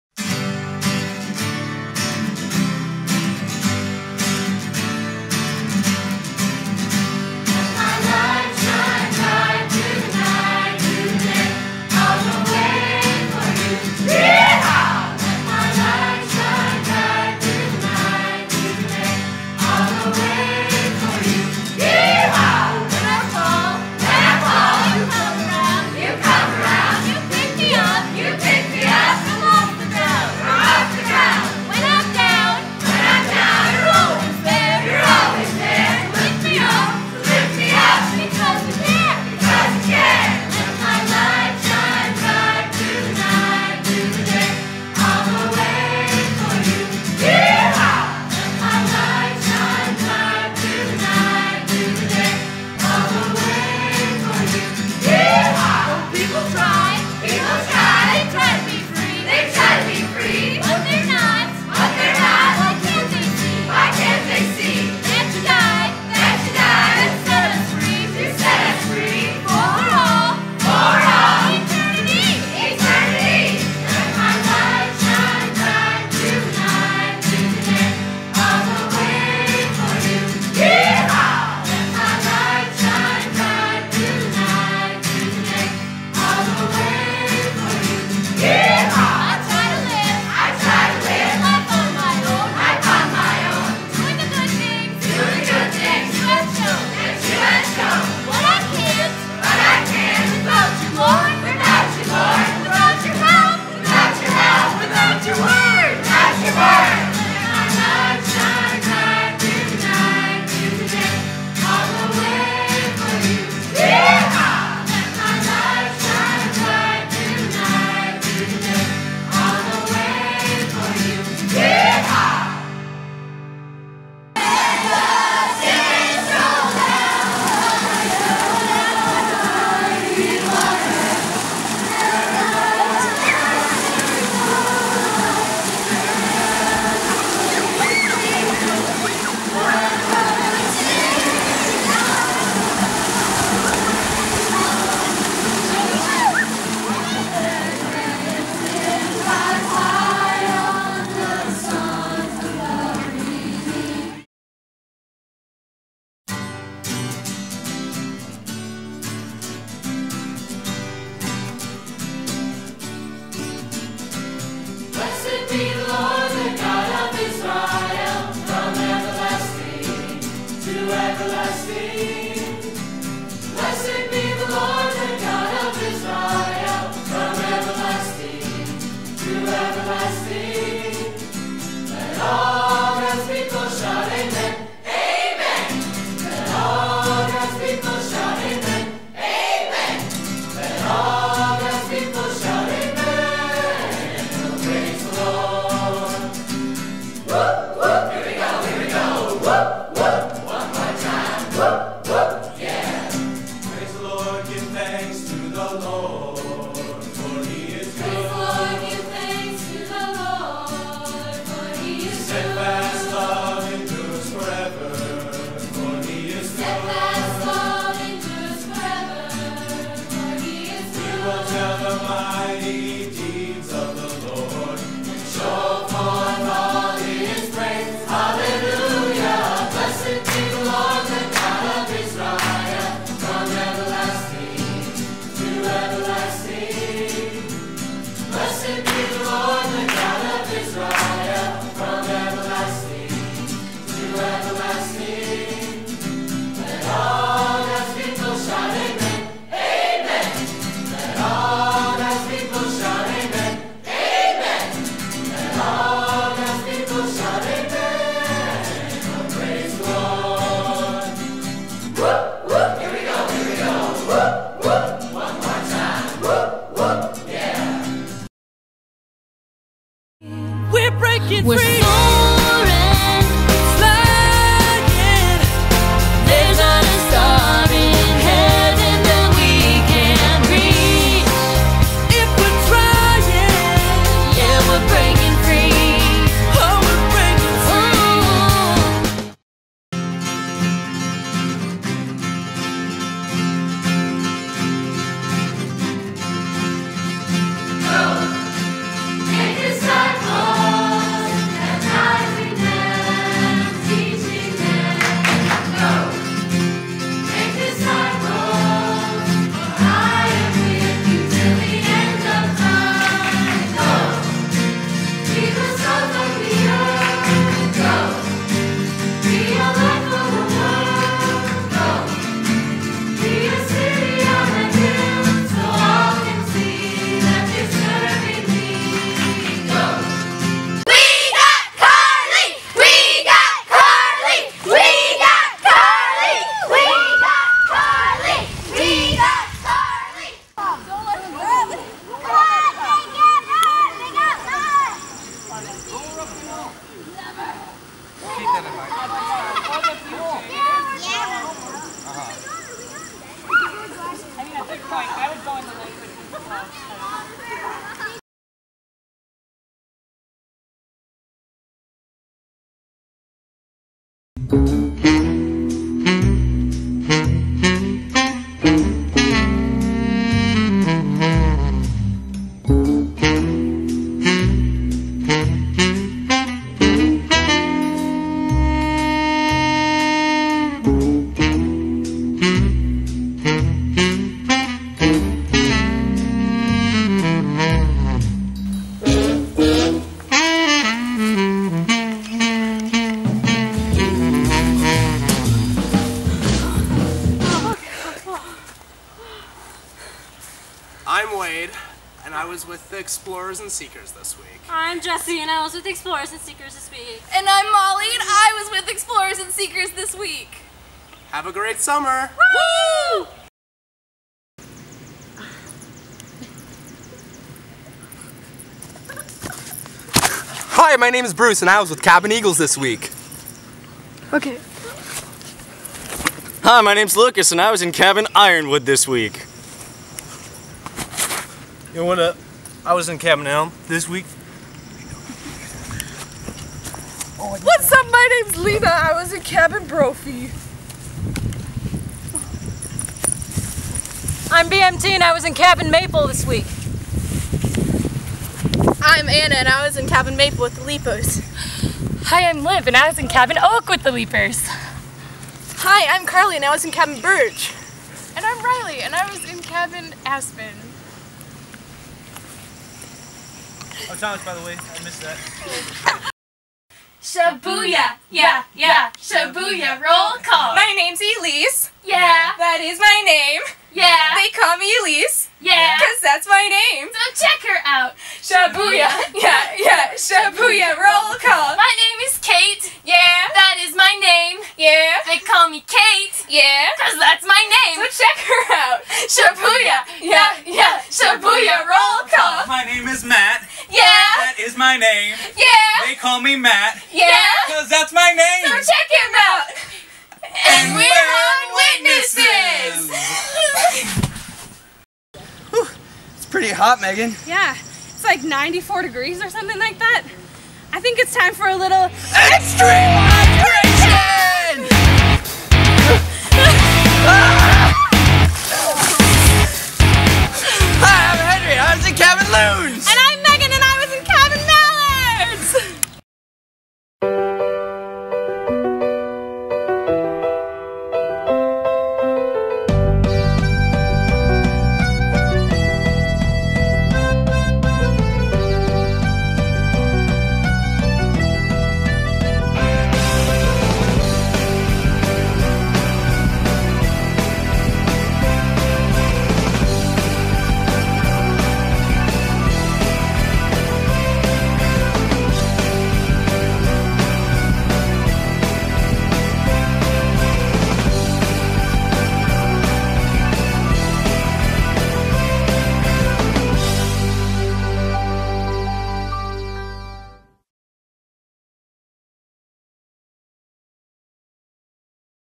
This Changes Everything-- July 8th to 13th 2018
This week at Luther Crest, we hosted Seekers, Explorers, Leapers, Witnesses, Xtreme, Blazers, and Wet & Wilderness! Take a look at all the fun and excitement we had as we looked into our summer theme, This Changes Everything!